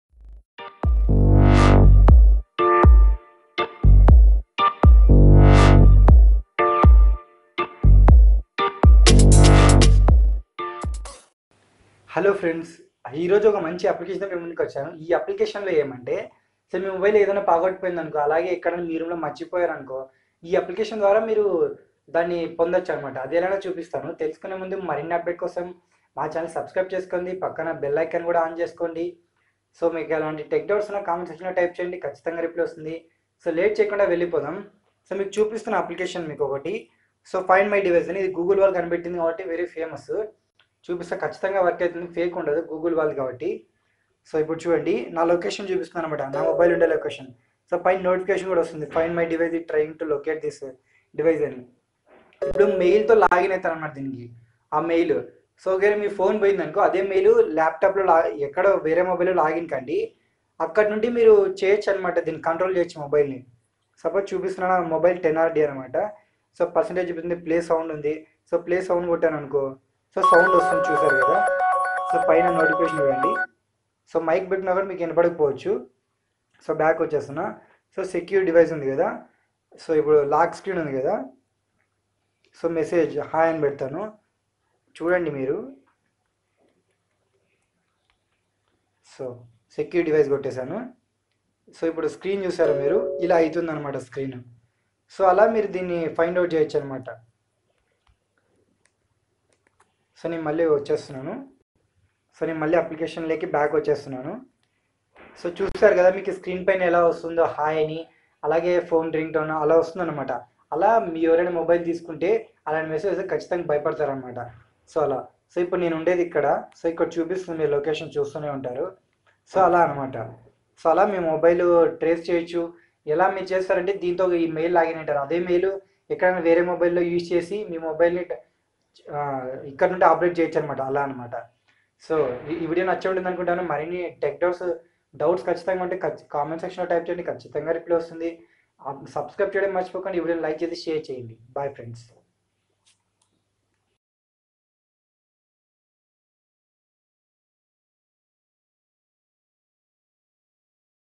How to trace mobile phone within one minute | find lost mobile | Telugu
In this video seen about how to know the location of stolen mobile..stolen phone.

VISIT OTHER SITES

stolen phone
mobile location track
mobile stolen